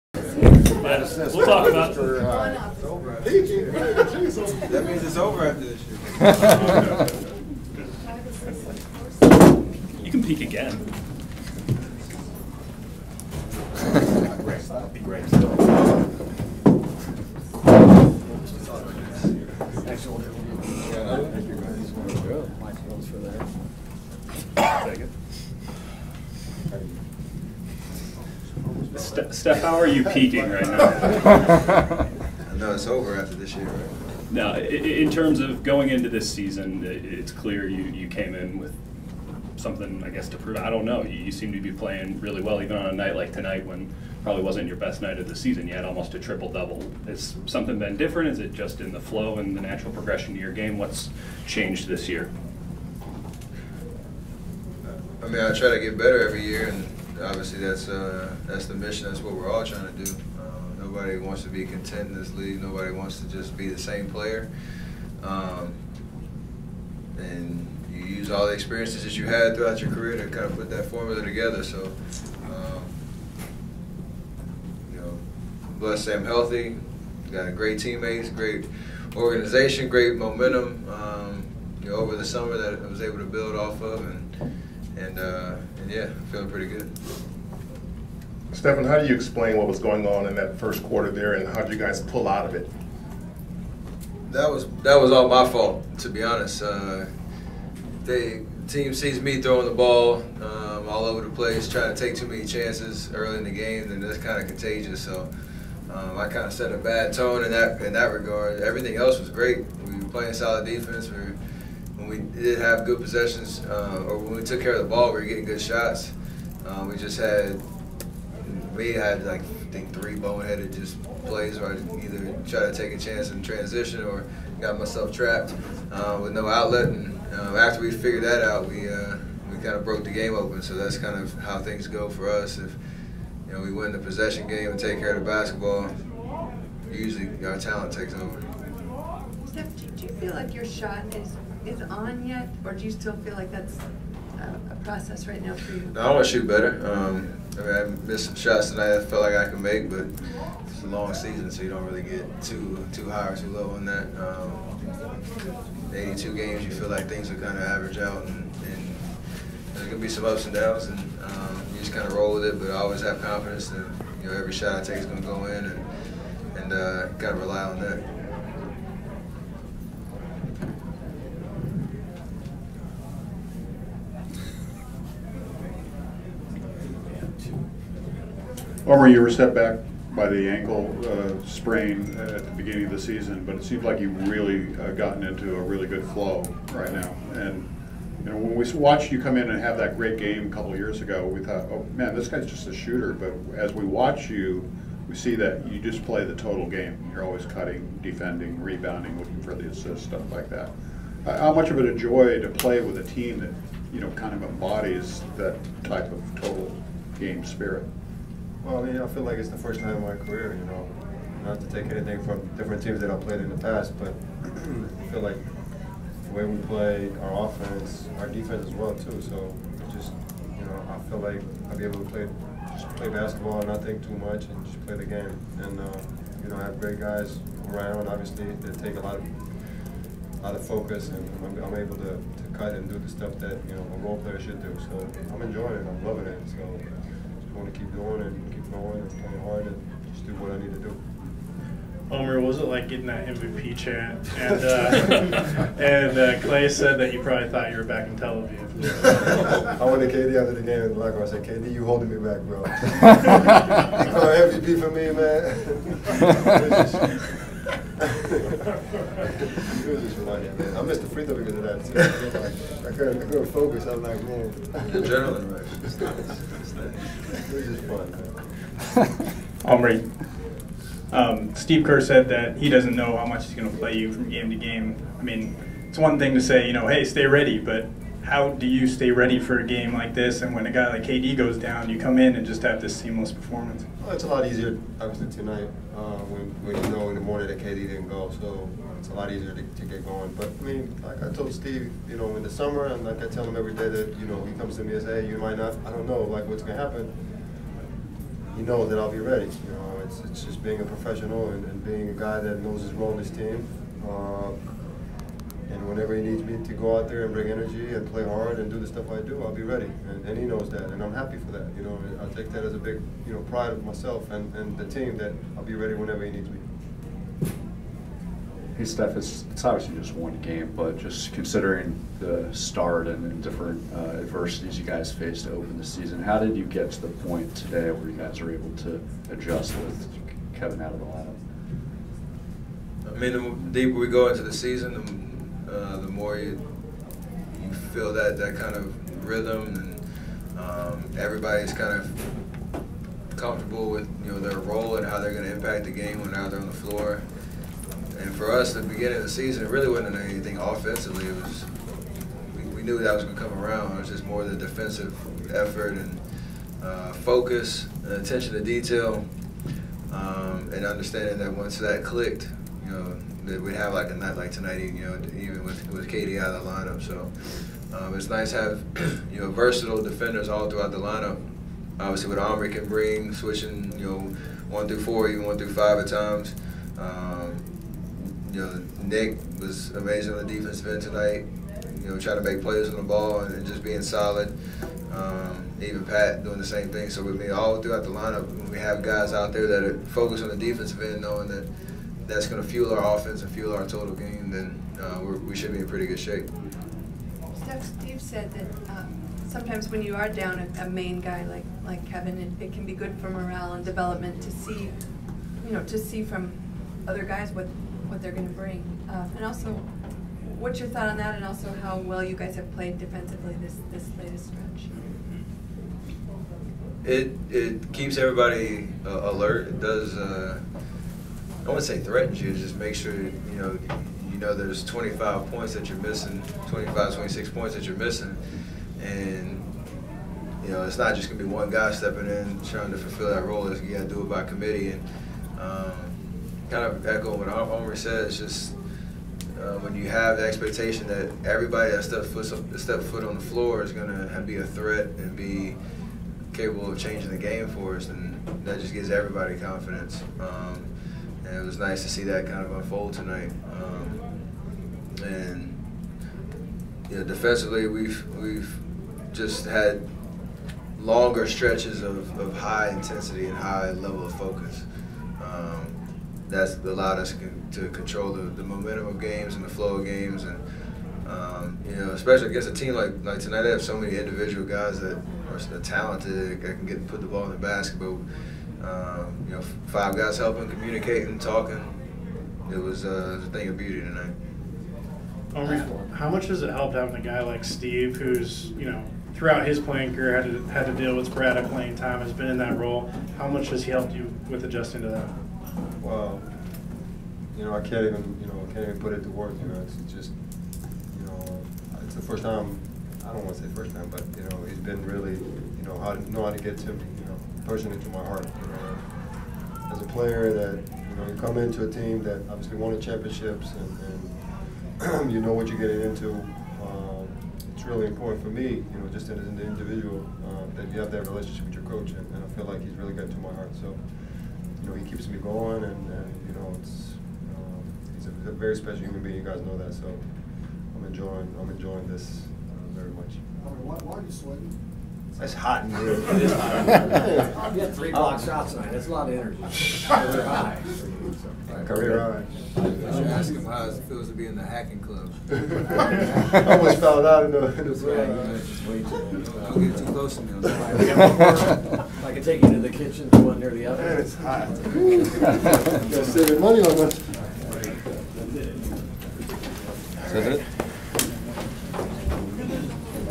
STEPH CURRY and OMRI CASSPI, postgame GSW (9-3) vs MIN: peaking?, turnovers, Casspi role
Next video: Iguodala postgame locker room